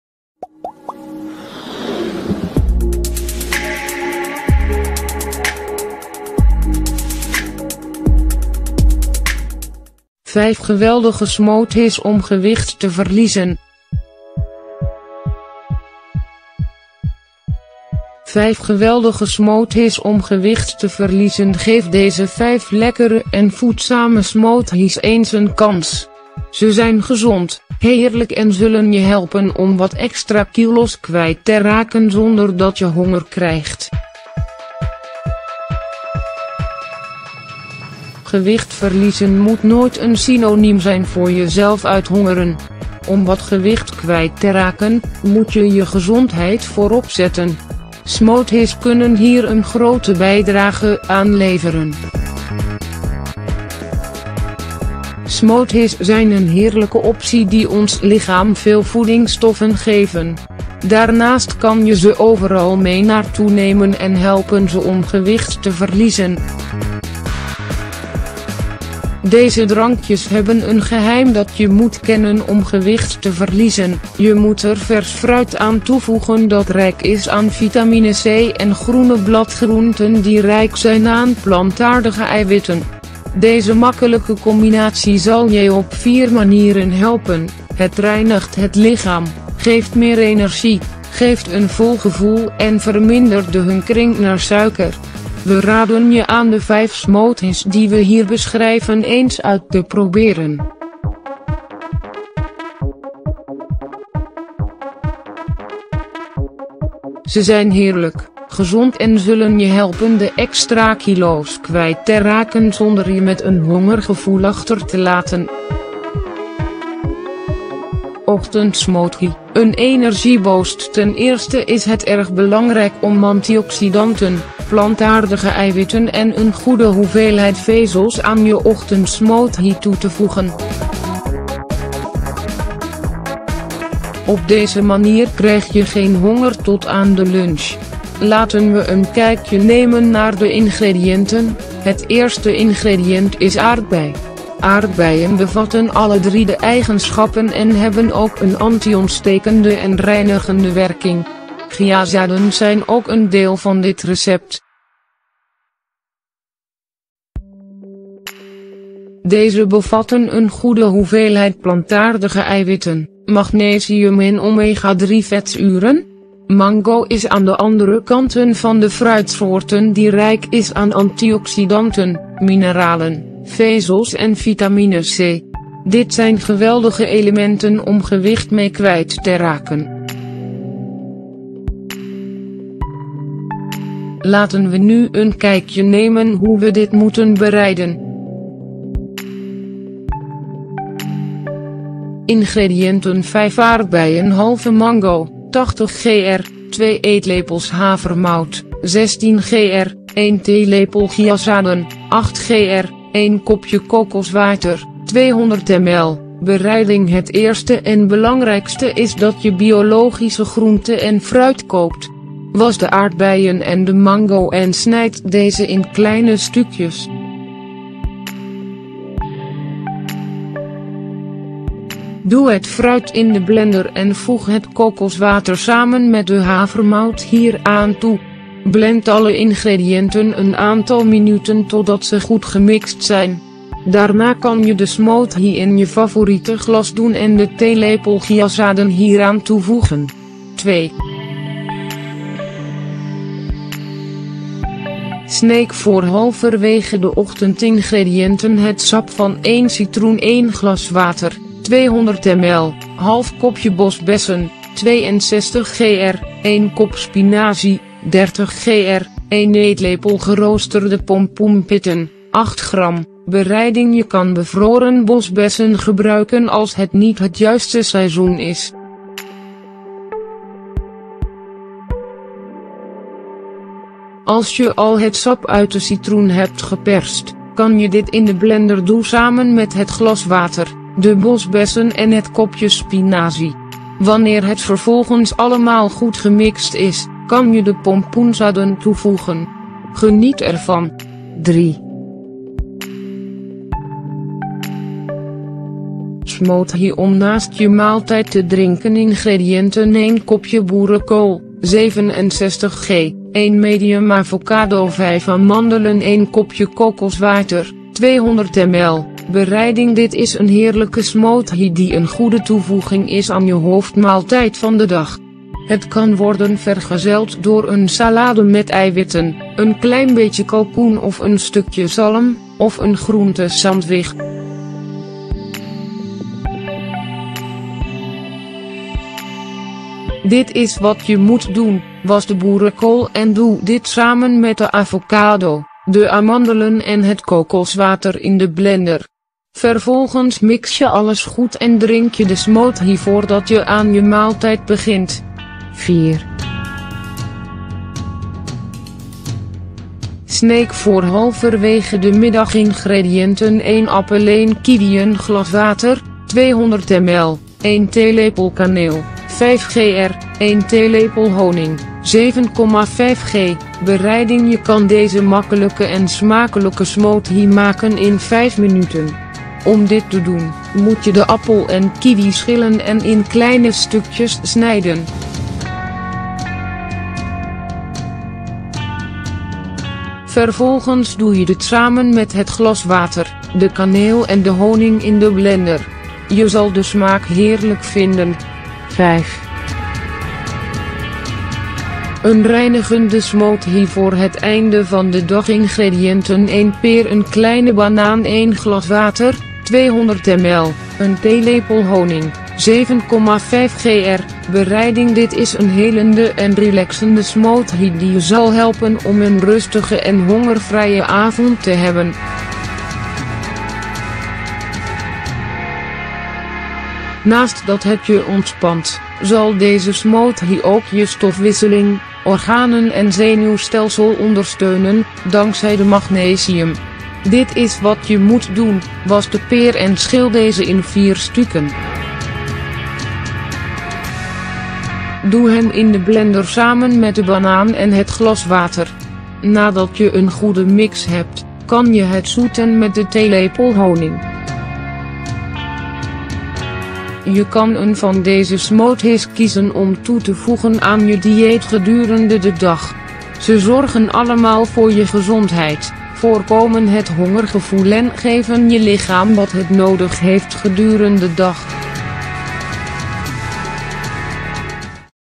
5 geweldige smoothies om gewicht te verliezen
5 geweldige smoothies om gewicht te verliezen, Geef deze vijf lekkere en voedzame smoothies eens een kans. Ze zijn gezond, heerlijk en zullen je helpen om wat extra kilo's kwijt te raken zonder dat je honger krijgt.